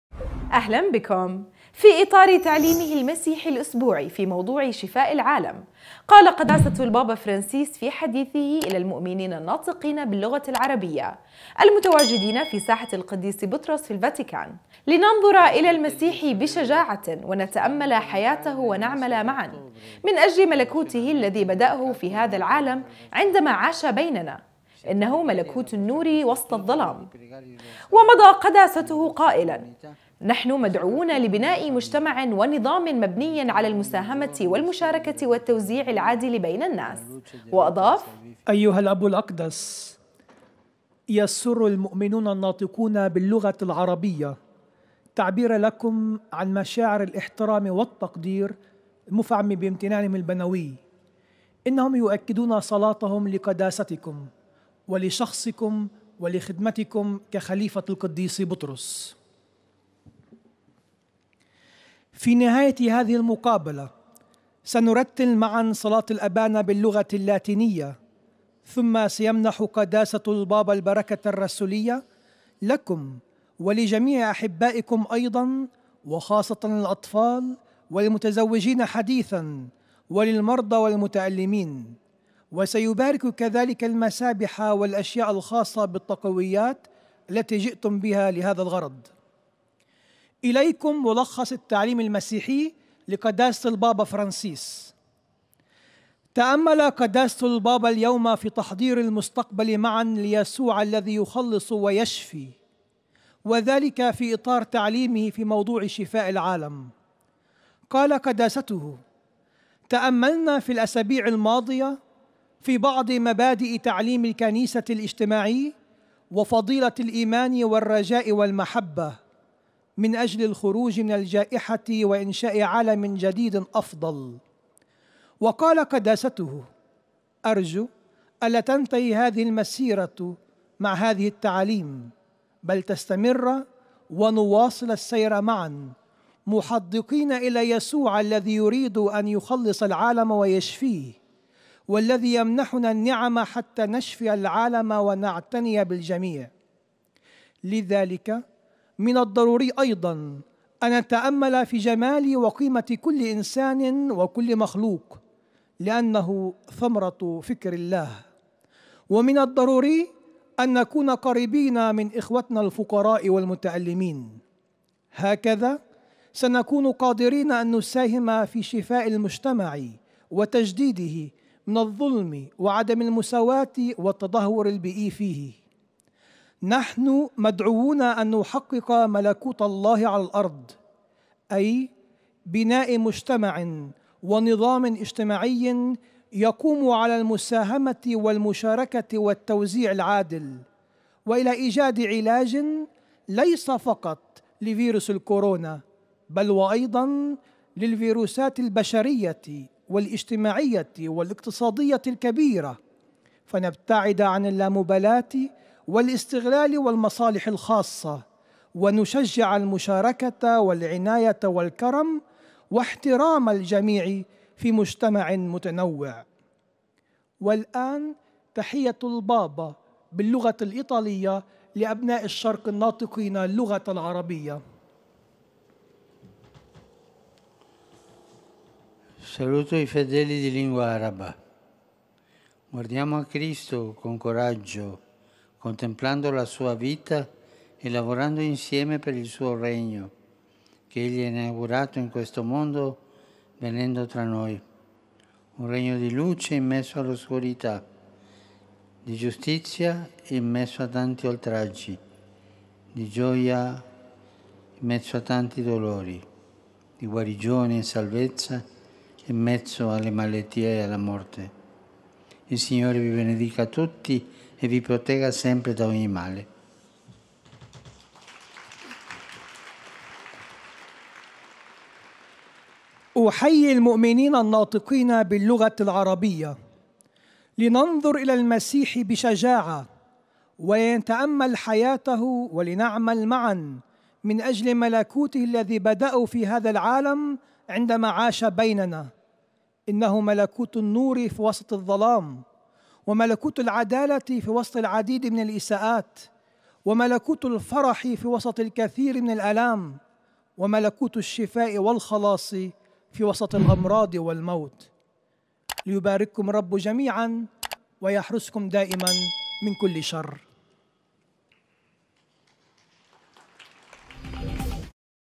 نورسات الأردن : البابا فرنسيس يدعونا للنظرِ والتأملِ في حياة المسيح إنهُ ملكوتُ النورِ وسطَ الظلام
في إطارِ تعليمهِ المسيحي الإسبوعي / في موضوعِ  شفاءِ العالم / قال قداسةُ البابا فرنسيس في حديثهِ الى المؤمنينَ الناطقينَ باللغةِ العربية / المتواجدينَ في ساحة القديس بطرس في الفاتيكان / لننظرَ الى المسيحِ بشجاعةٍ ونتأملَ حياتَه ونعملَ معاً / من أجلِ ملكوتهِ الذي بدأهْ في هذا العالم عندما عاشَ بيننا / إنه ملكوتُ النورِ وسطَ الظلام / ومضى قداستُه قائلا / نحن مدعوونَ لبناءِ مجتمعٍ ونظامٍ مبنيٍ على المساهمةِ والمشاركةِ والتوزيعِ العادلِ بين الناس / وأضاف